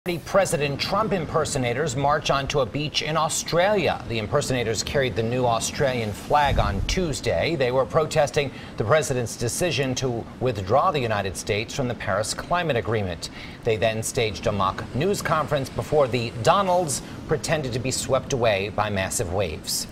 President Trump Impersonators Protest On Australian Beach
They were protesting the president's decision to withdraw the U.S. from the Paris Climate Agreement.